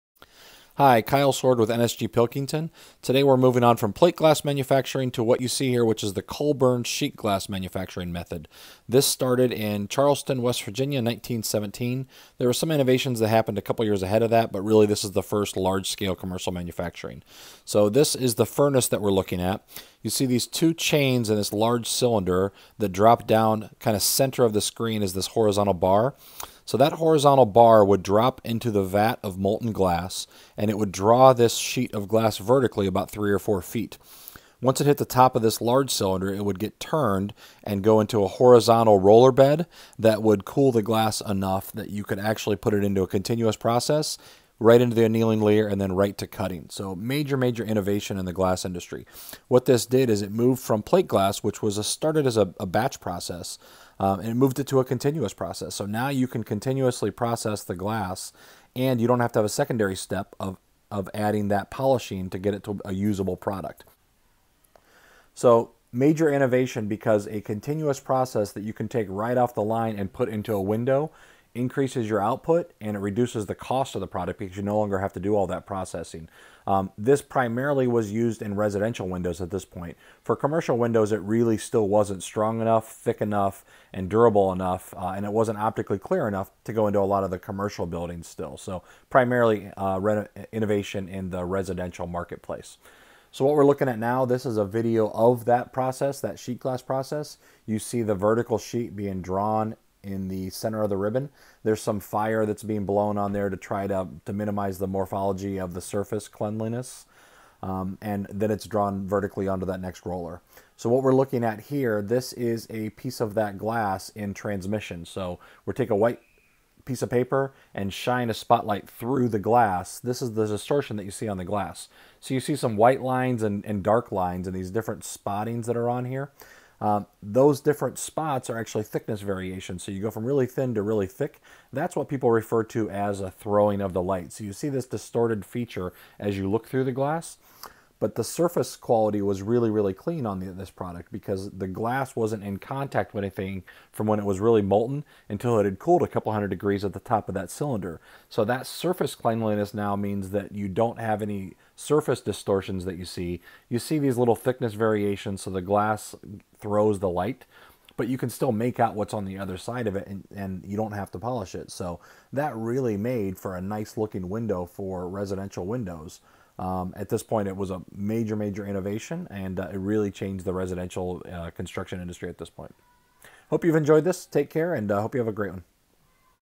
Sheet glass manufacturing
Video from LOF 1927.  Examines Colburn sheet glass manufacturing method.